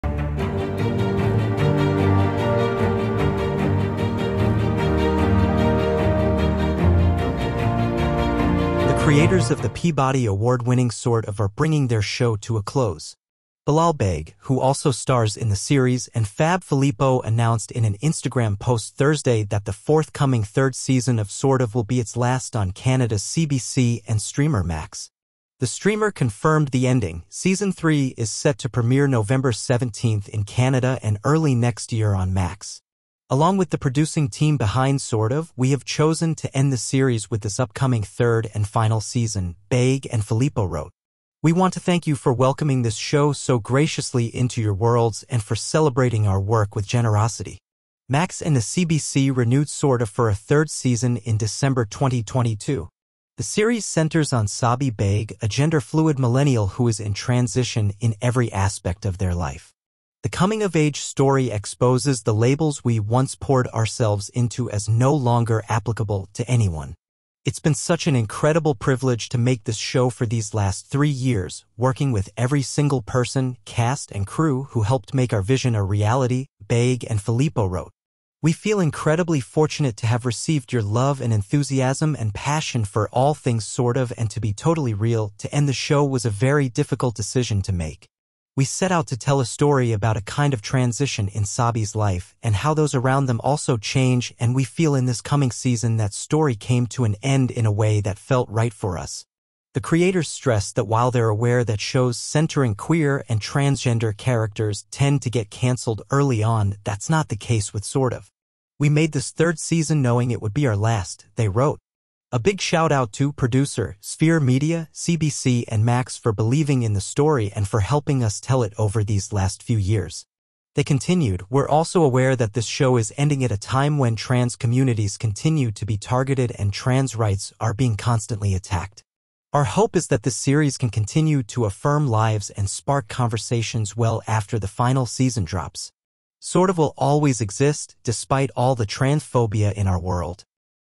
Sort Of’ to End With Season 3 on CBC, Max
The creators of the Peabody Award-winning Sort Of are bringing their show to a close.

Bilal Baig (who also stars in the series) and Fab Filippo announced in an Instagram post Thursday that the forthcoming third season of Sort Of will be its last on Canada’s CBC and streamer Max. The streamer confirmed the ending; season three is set to premiere Nov. 17 in Canada and early next year on Max.

“Along with the producing team behind Sort Of, we have chosen to end the series with this upcoming third and final season,” Baig and Filippo wrote. “We want to thank you for welcoming this show so graciously into your worlds and for celebrating our work with generosity.”